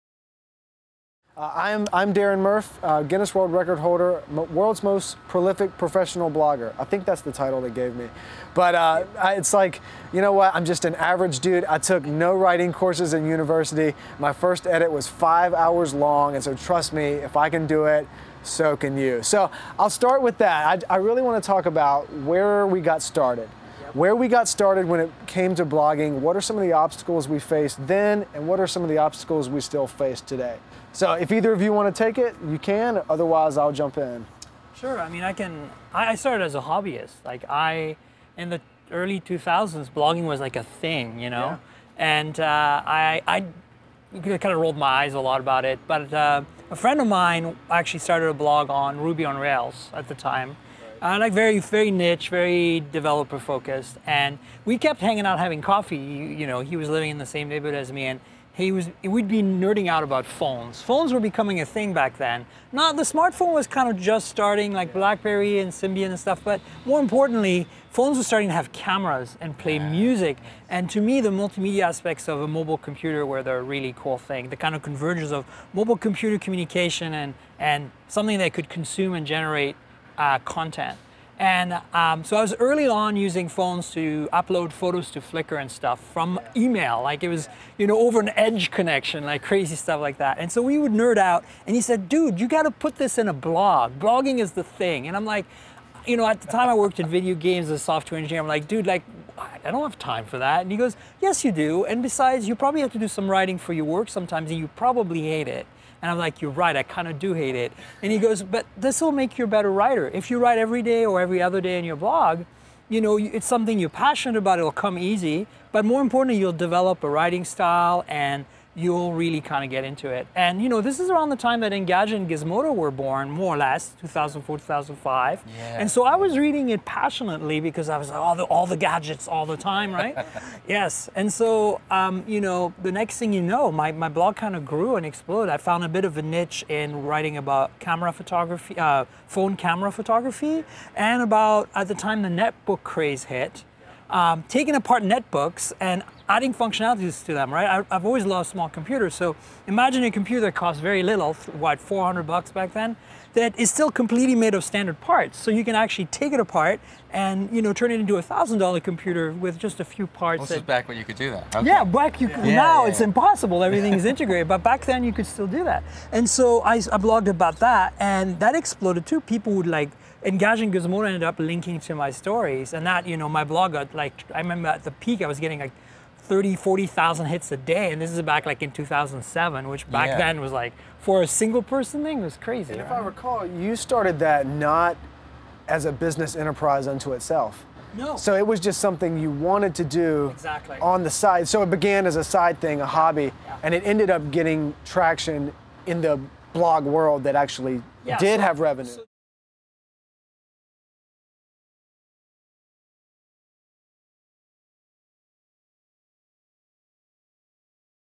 How Professional Bloggers Got Started: Myriam Joire
Myriam Joire, Founder of TNKGRL Media and Sean Hollister, Senior Editor at CNET join Darren Murph who holds the Guinness World Record® for being the most prolific professional blogger, to talk about how they got started blogging and give tips to those that are aspiring bloggers. Check out Darren's half-day CreativeLive class How to Write a Blog that Drives Traffic